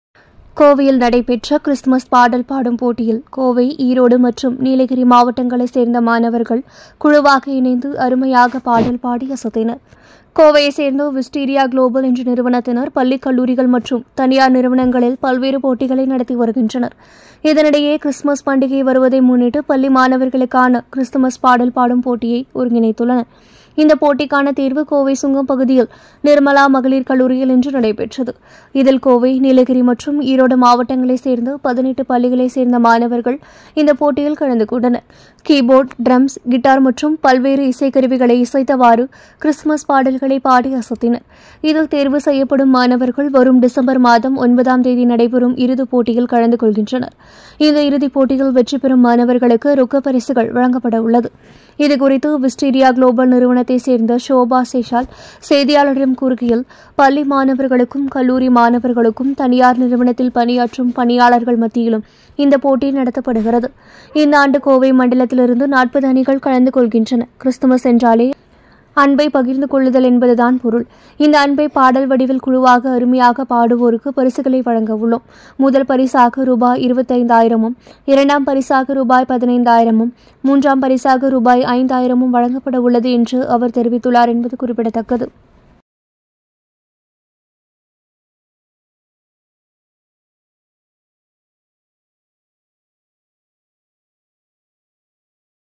கோவையில் நடைபெற்ற கிறிஸ்துமஸ் பாடல் பாடும் போட்டியில் கோவை, ஈரோடு மற்றும் நீலகிரி..
கோவையை சேர்ந்த விஸ்டீரியா க்ளோபல் என்ற நிறுவனத்தினர் பள்ளி கல்லூரிகள் மற்றும் தனியார் நிறுவனங்களில் பல்வேறு போட்டிகளை நடத்தி வருகின்றனர். இதனிடையே கிறிஸ்துமஸ் பண்டிகை  வருவதை முன்னிட்டு பள்ளி மாணவர்களுக்கான கிறிஸ்துமஸ் பாடல் பாடும் போட்டியை ஒருங்கிணைத்துள்ளனர் இந்த போட்டிக்கான தேர்வு கோவை சுங்கம் பகுதியில், நிர்மலா மகளிர் கல்லூரியில் இன்று  நடைபெற்றது.
இதில், கோவை, நீலகிரி மற்றும் ஈரோடு மாவட்டங்களை சேர்ந்த 18 பள்ளிகளை சேர்ந்த மாணவர்கள் இந்த போட்டியில் கலந்து கொண்டனர்.
கீபோர்ட், டிரம்ஸ், கித்தார் உள்ளிட்ட வெவ்வேறு இசைக்கருவிகளை இசைத்தவாறு கிறிஸ்துமஸ் பாடல்களை பாடி அசத்தினர். இதில் தேர்வு செய்யப்படும் மாணவர்கள் வரும் டிசம்பர் மாதம் 9ம் தேதி நடைபெறும் இறுதி போட்டியில் கலந்து கொள்கின்றனர். இந்த இறுதி போட்டியில் வெற்றி பெறும் மாணவர்களுக்கு ரொக்க பரிசுகள் வழங்கப்பட உள்ளது.
இதுகுறித்து விஸ்டீரியா க்ளோபல் நிறுவனத்தை சேர்ந்த ஷோபா சேஷல் செய்தியாளர்களிடம் கூறுகையில், "பள்ளி மாணவர்களுக்கும், கல்லூரி மாணவர்களுக்கும், தனியார் நிறுவனத்தில் பணியாற்றும் பணியாளர்கள் மத்தியிலும் இந்த போட்டி நடத்தப்படுகிறது. இந்தாண்டு கோவை மண்டலத்தில் இருந்து 40 அணிகள் கலந்து கொள்கின்றன. கிறிஸ்துமஸ் என்றாலே அன்பை பகிர்ந்து கொள்ளுதல் என்பது தான் பொருள். இந்த அன்பை பாடல் வடிவில் குழுவாக அருமையாக பாடுவோர்க்கு பரிசுகளை வழங்க உள்ளோம். முதல் பரிசாக ரூ.25 ஆயிரமும், இரண்டாம் பரிசாக ரூ.15 ஆயிரமும் மூன்றாம் பரிசாக ரூ.5 ஆயிரமும் வழங்கப்பட உள்ளது." என்றார் என்பது குறிப்பிடத்தக்கது